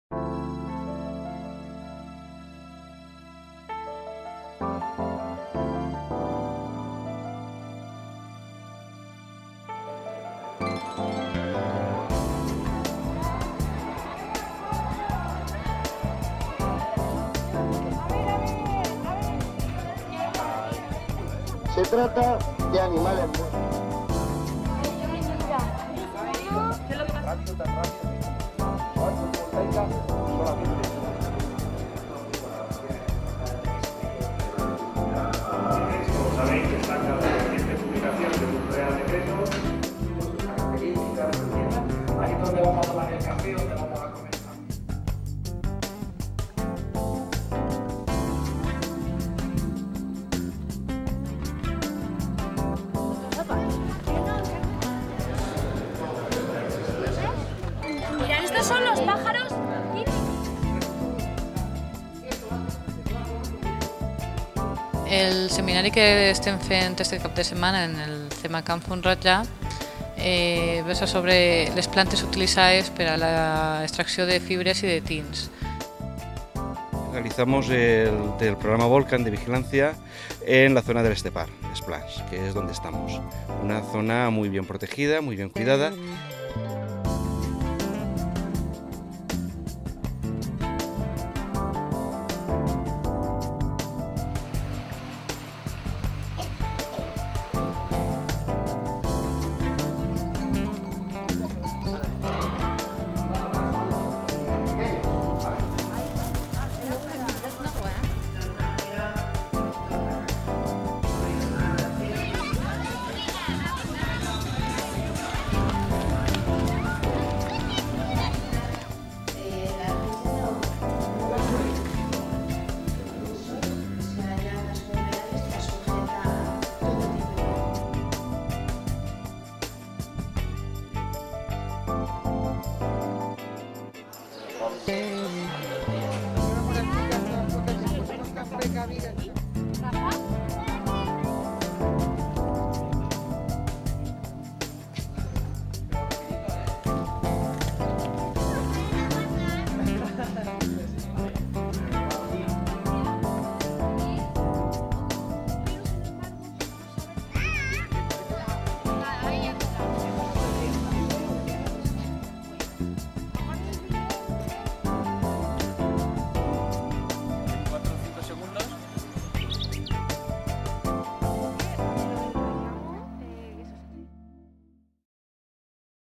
Centros de Medio Ambiente CAM - Obra Social Caja Mediterráneo
Centros de Medio Ambiente Caja Mediterráneo, ejemplos de construcciones sostenibles. Se organizan encuentros sobre cambio climático, gestión ambiental o conservación. Seminarios y jornadas divulgativas promocionan el cuidado del medio ambiente. Acciones de voluntariado ambiental promueven la recuperación del entorno. El visitante puede seguir cursos de educación medioambiental o asomarse al universo a través de un observatorio astronómico.